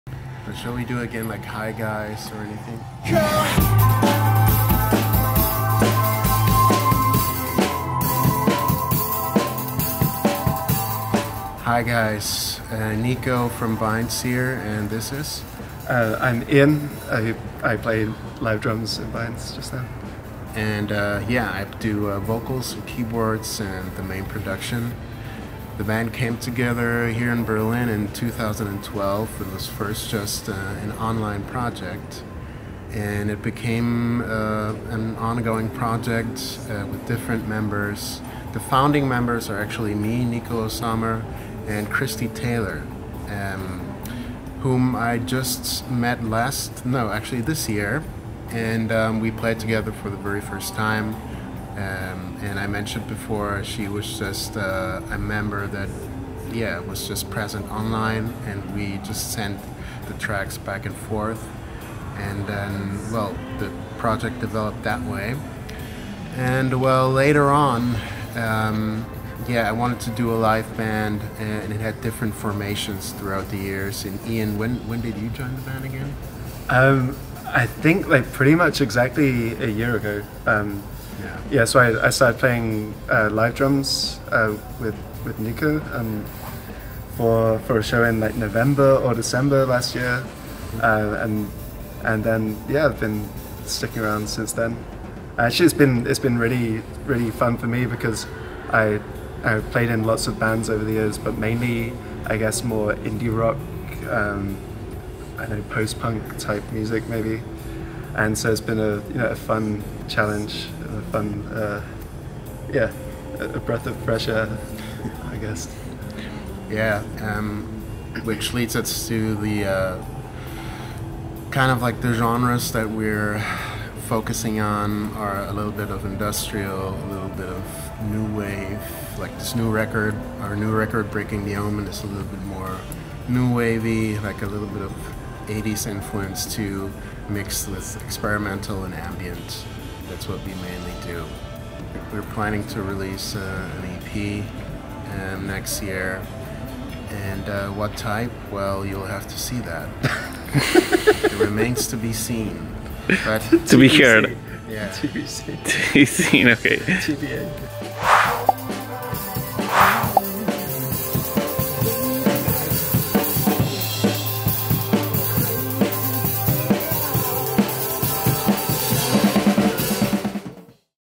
BIINDS at Kiremico Interviews - Berlin Noisy Rooms
Just days before the performance of BIINDS at KAOS Krystal Maze Party, we took a moment to interview and record this amazing band at Noisy Room Studios in Berlin.